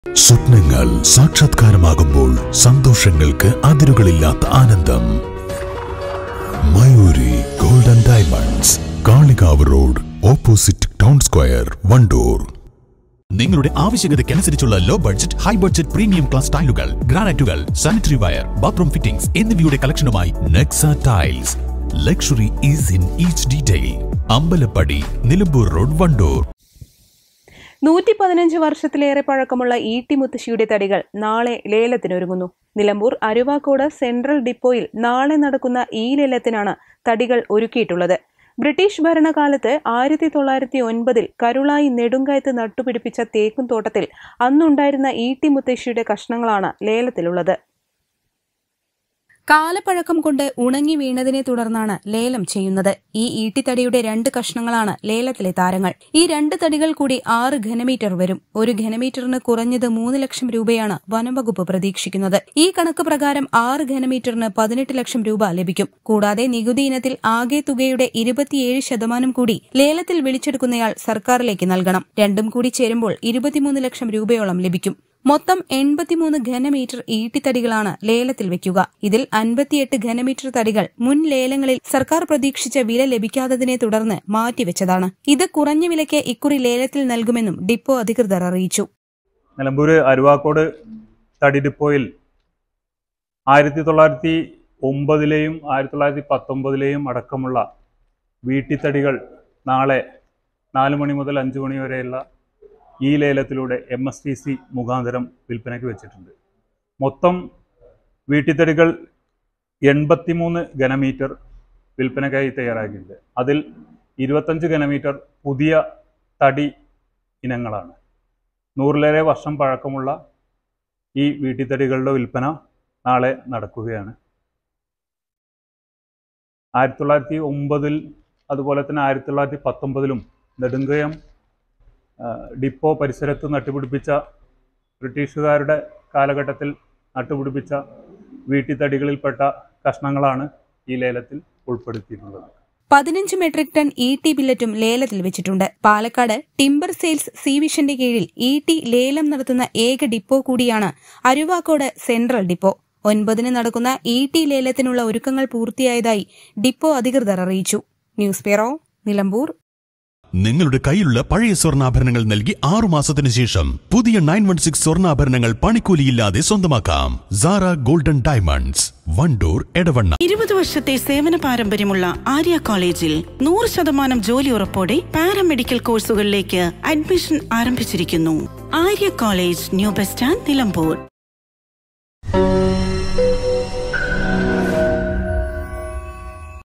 115 വർഷത്തിലേറെ പഴക്കമുള്ള ഇട്ടിത്തടികൾ ലേലത്തിന്
നാളെയാണ് ലേലം